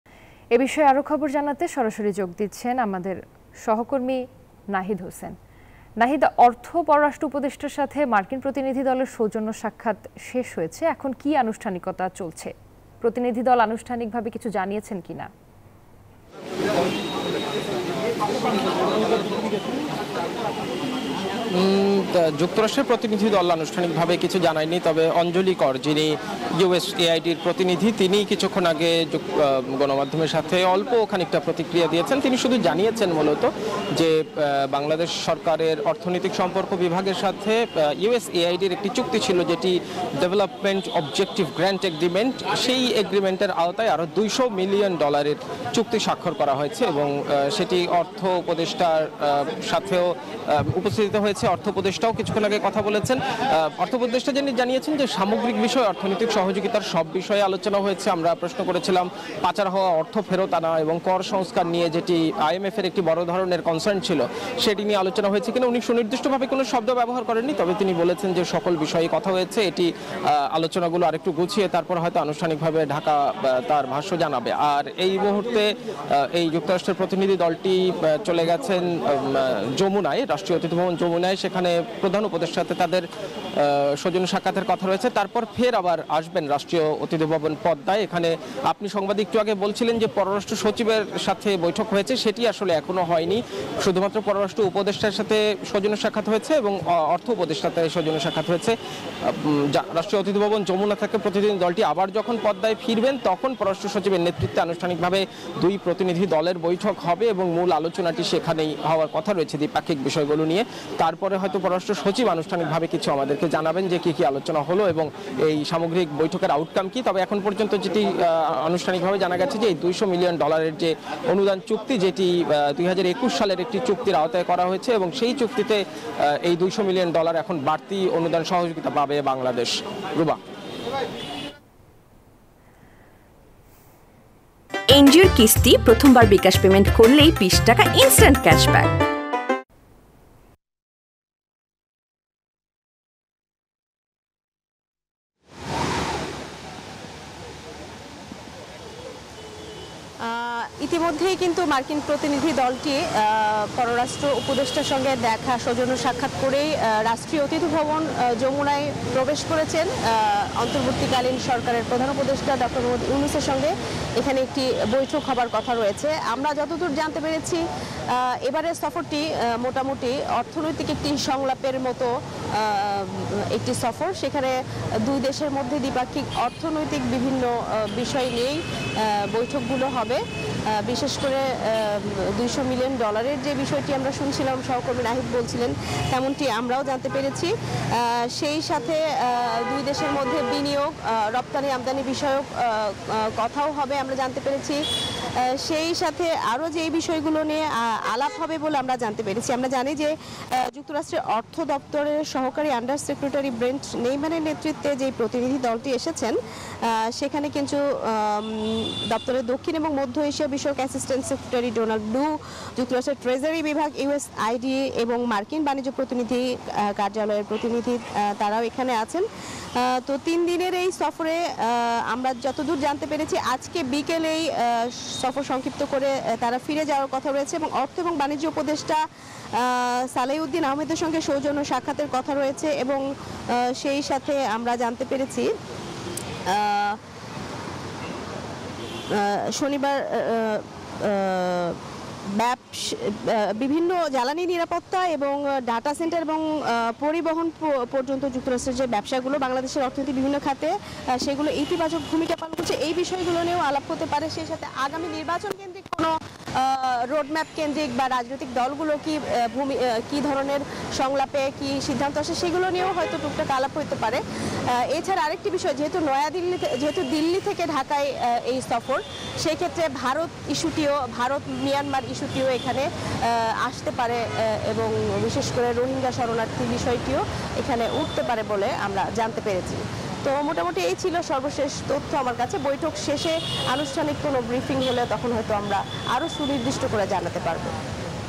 অর্থ ও পররাষ্ট্র উপদেষ্টার সাথে মার্কিন প্রতিনিধি দলের সৌজন্য সাক্ষাতের সবশেষ | Us Meeting | News24
Our other YouTube channels :
News24 Official Address:
NEWS24
371/A
Bashundhara
Block – D
Bashundhara Residential Area
Dhaka - 1229